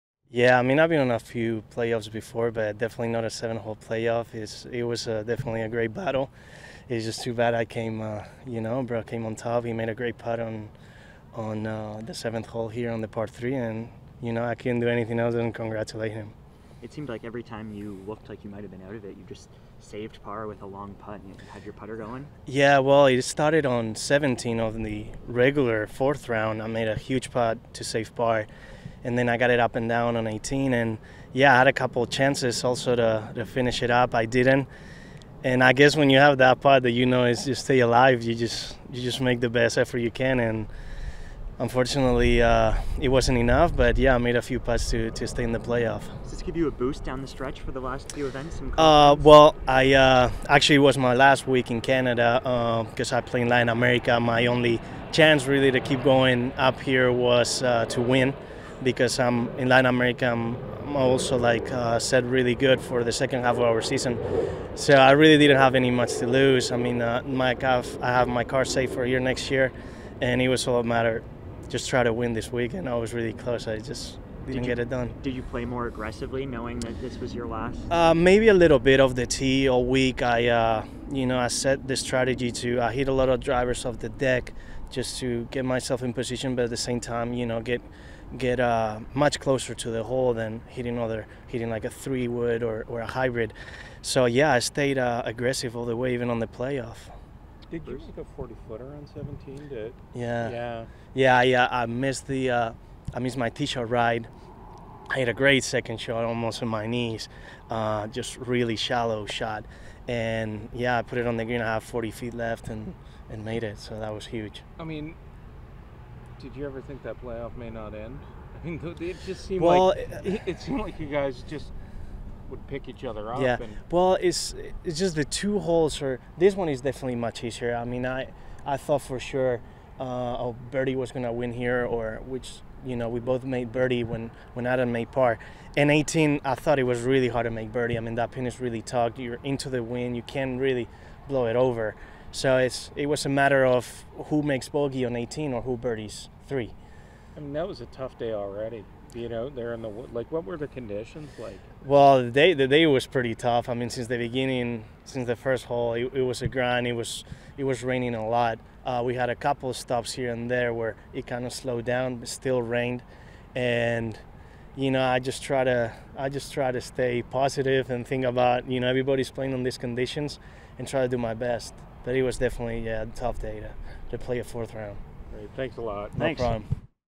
Samuel Del Val interview after Round 4 of National Capital Open
Following his final-round 70 at the 2016 News Sentinel Open presented by Pilot, Samuel Del Val talks about keeping a hot putter throughout the day.